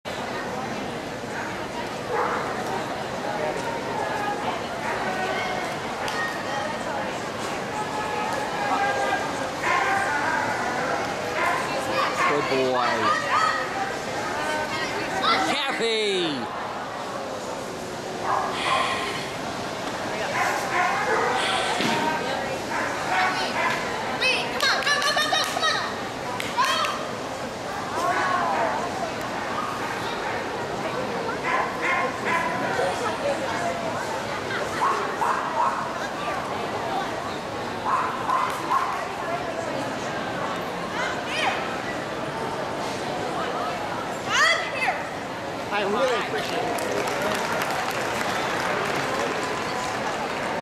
Austin 2010 AKC Invitational Standard Rd 1
Austin's first Standard run at the 2010 AKC Agility Invitational in Long Beach, CA.  Can we just pretend the off-course never happened?  Such a nice run otherwise.